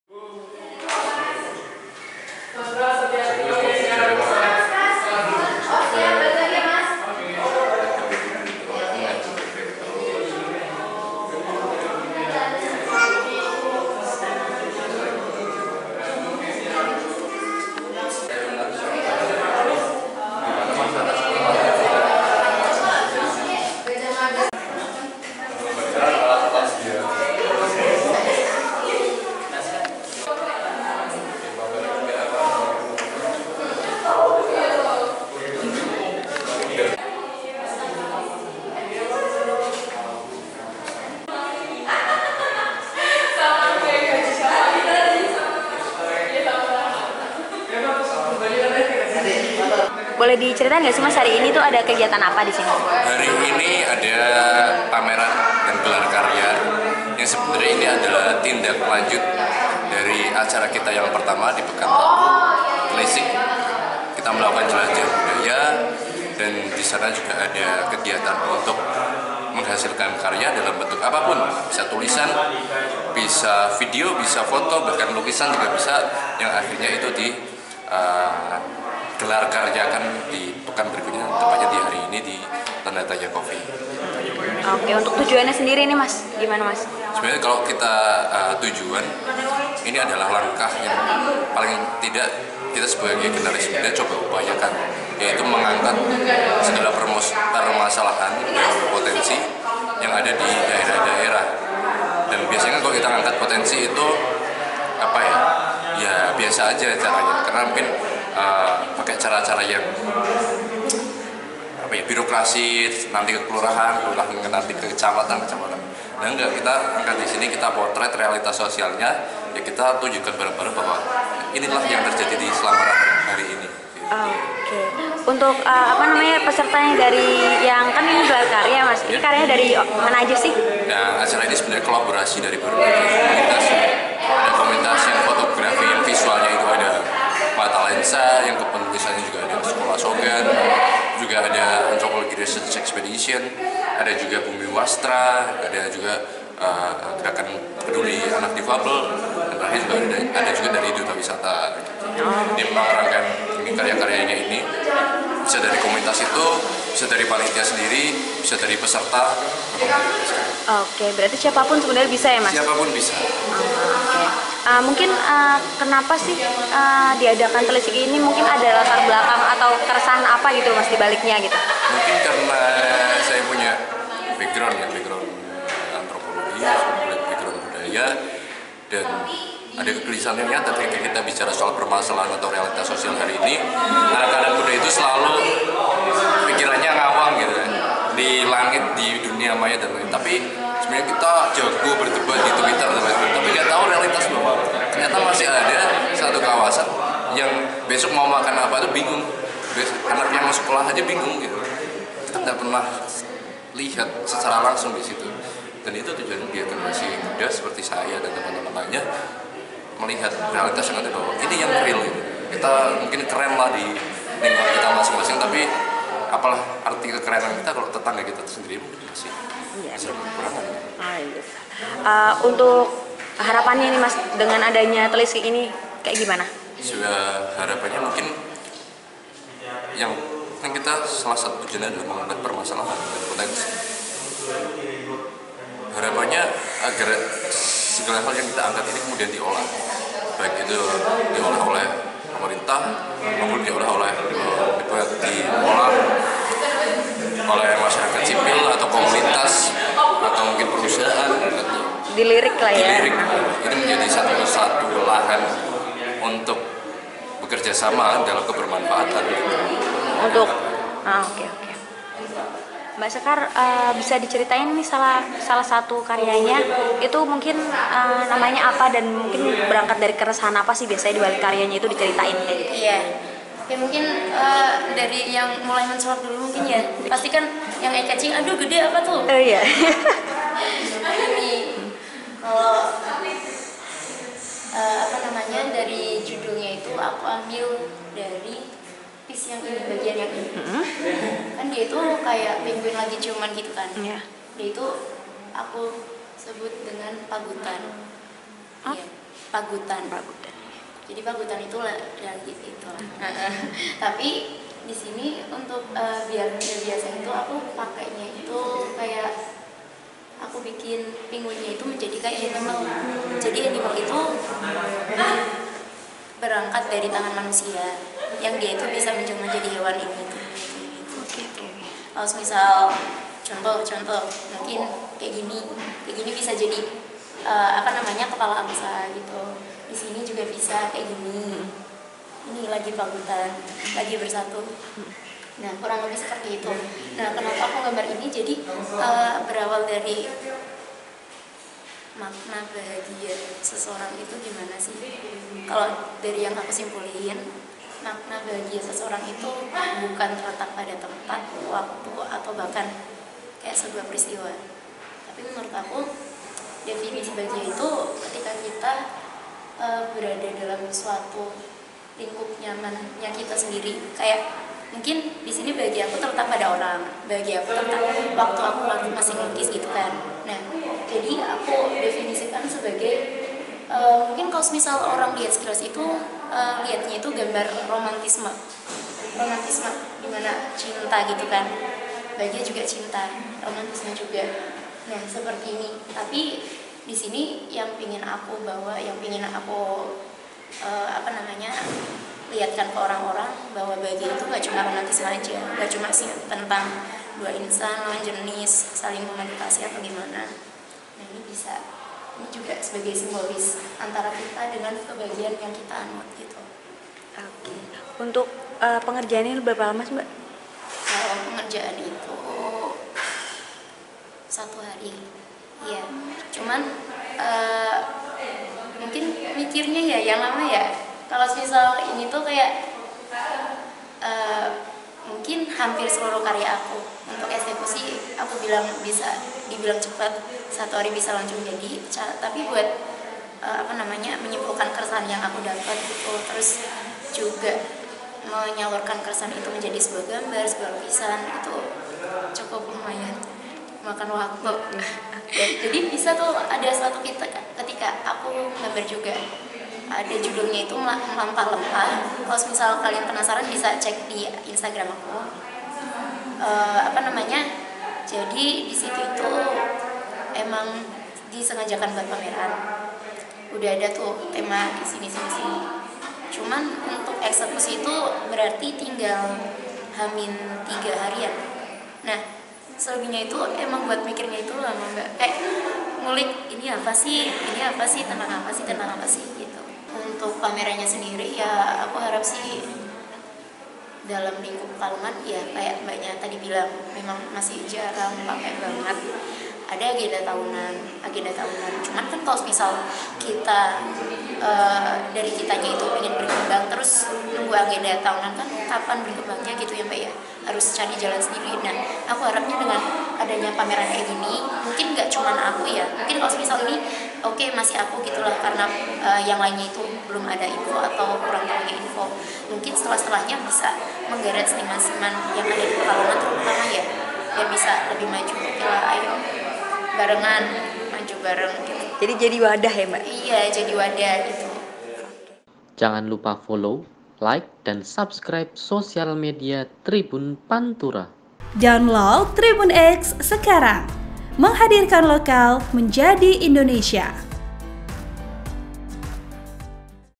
Ada Pameran Seni Menelisik Realitas Sosial di Pantai Slamaran Pekalongan
Belasan karya seni dari seniman di Pekalongan tergabung dalam berbagai komunitas dipamerkan dalam pameran Telisik Vol.1: Slamaran yang bertema "Berkelana bersama menggali makna budaya" di Kafe Tanda Tanya Coffee.

Pameran ini tindak lanjut kegiatan menelisik beberapa area di Pantai Slamaran Pekalongan pada 4 Februari.

Pameran ini merupakan langkah mengangkat permasalahan dan potensi di daerah dengan memotret realita sosial.

Diharapkan pemerintah bisa melirik daerah tersebut dan melakukan perubahan lebih baik.

Editor: edr